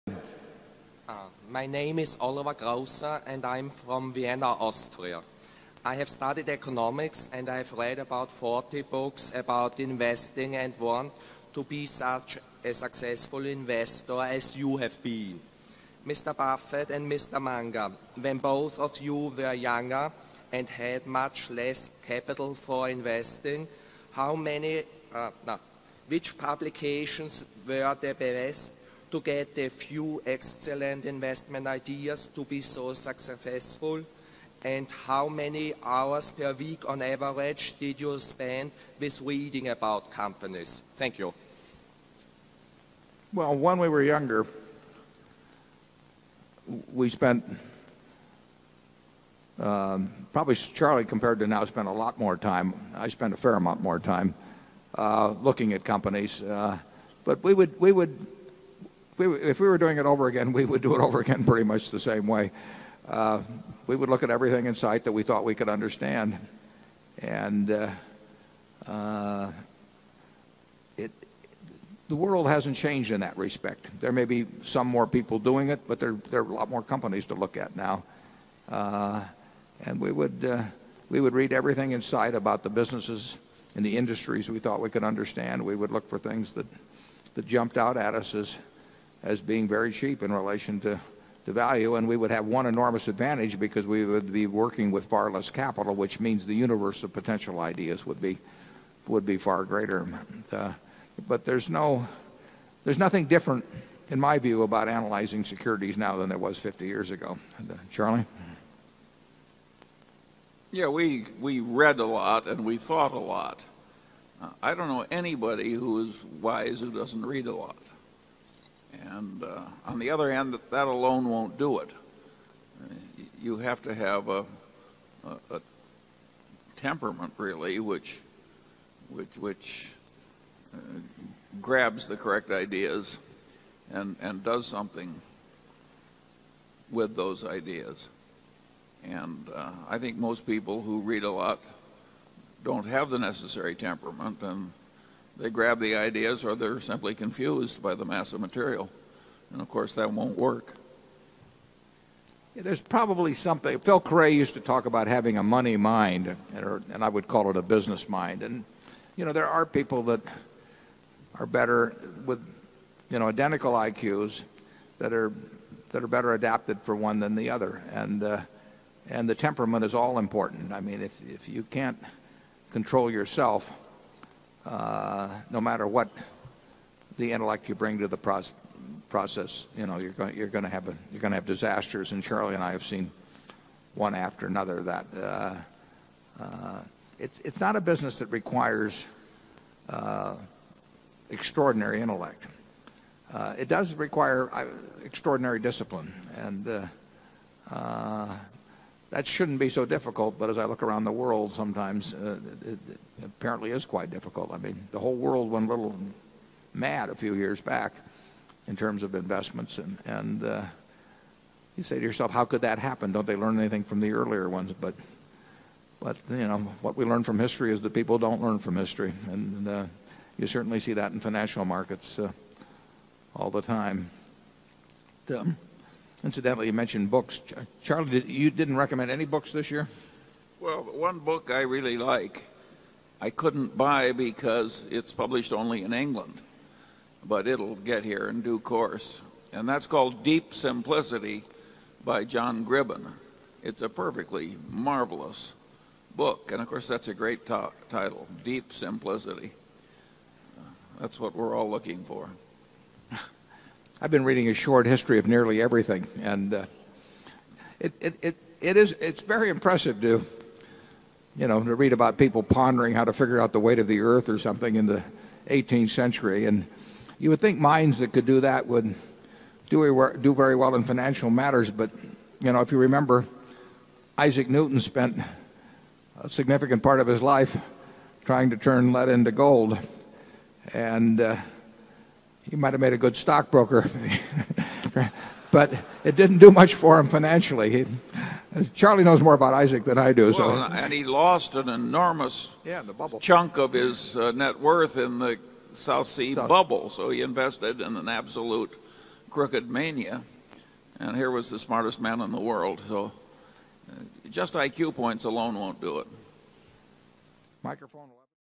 Warren Buffett & Charlie Munger both recommend a book at the 2004 annual meeting
Deep Simplicity by John Gribbin

A Short History of Nearly Everything by Bill Bryson